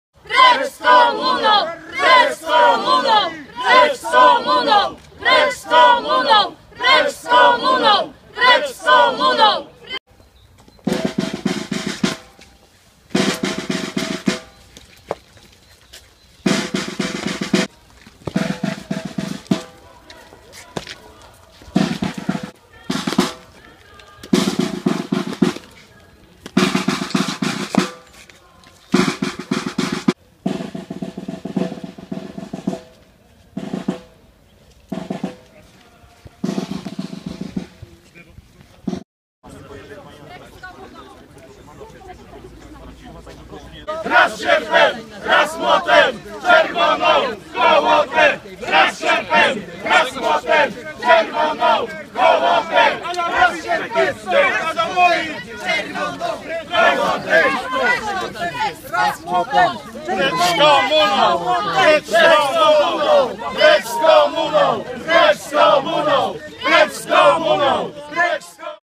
Protest na Cmentarzu Żołnierzy Radzieckich podczas 70. rocznicy zdobycia Gdańska.
27 marca 2015, protest przeciwko uroczystościom związanym z 70. rocznicą zdobycia Gdańska na cmentarzu, gdzie pochowani są żołnierze Armii Czerwonej.